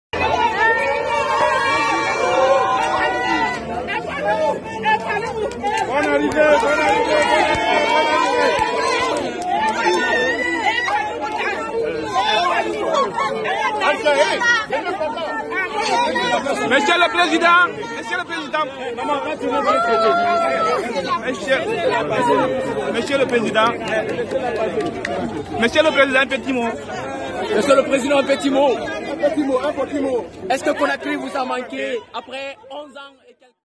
Dadis Camara à Conakry
Moussa Dadis Camara, rentré aujourd’hui d’un exil de onze ans